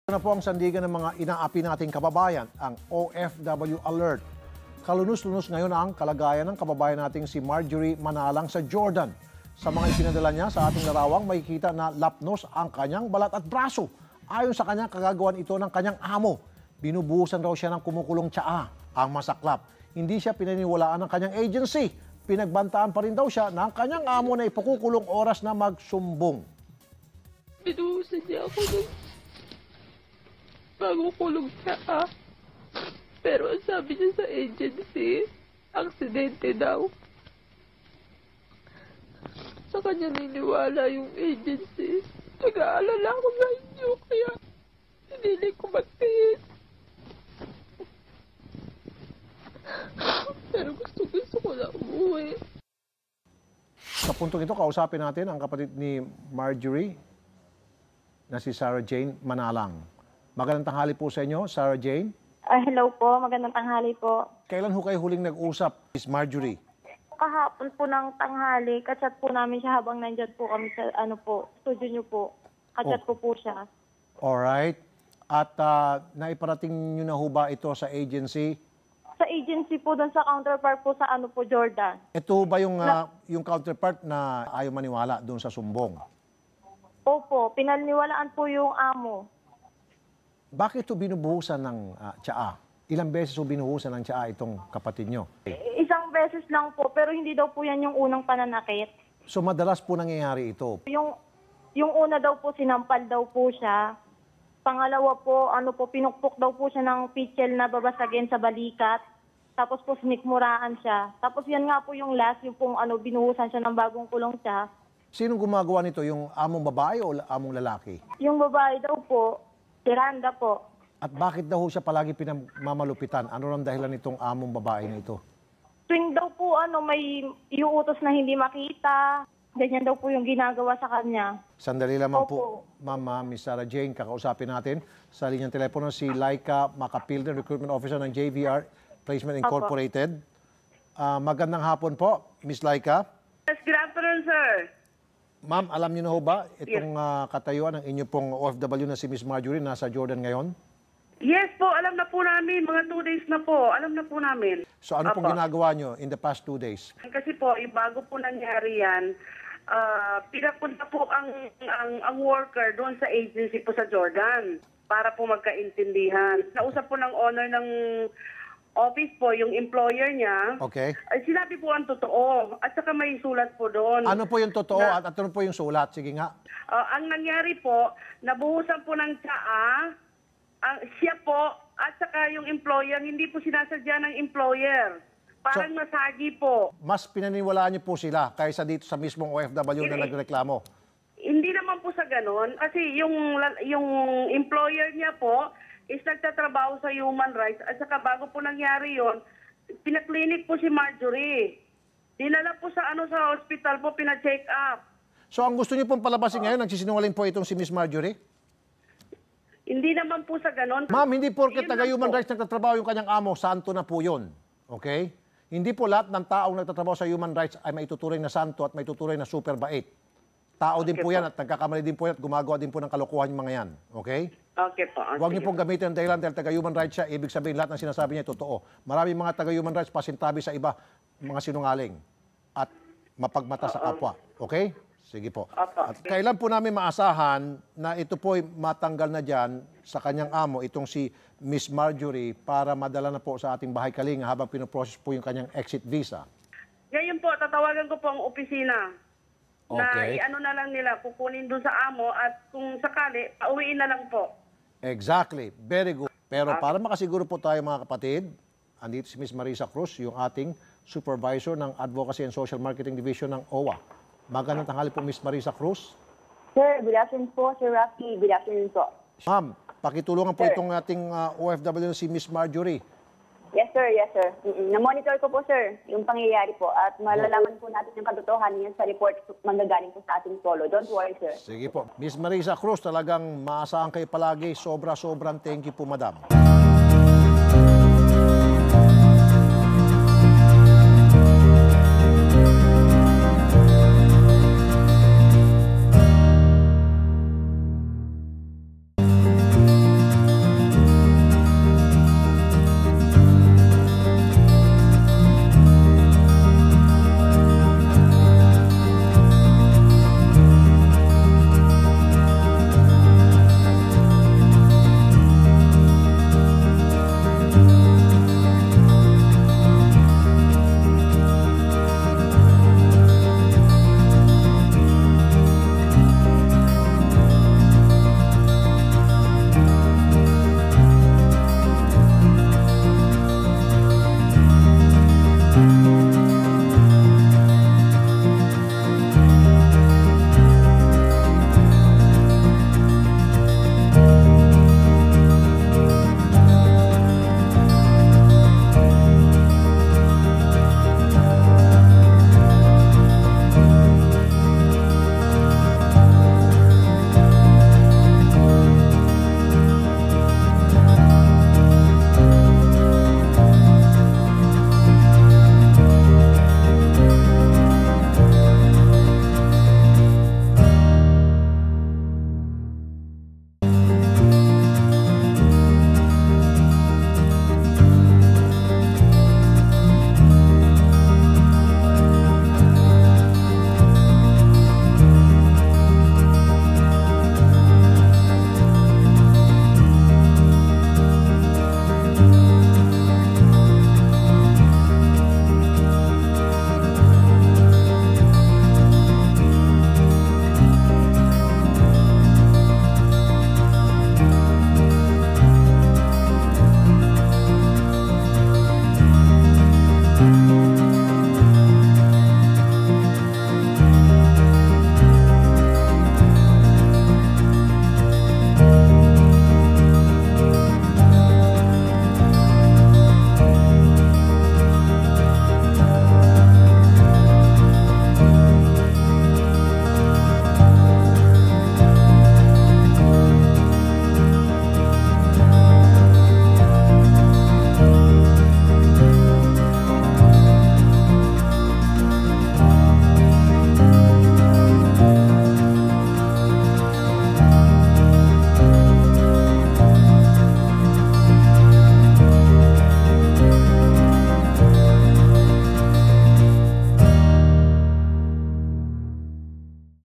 OFW SA JORDAN PINAGINITAN NG AMONG BABAE
OFW sa Jordan pinaginitan ng among babae. Sinampal, sinikmuraan at pinukpok ng pitchel.

Hanggang sa binuhusan ng mainit na tsaa at malapnos ang braso.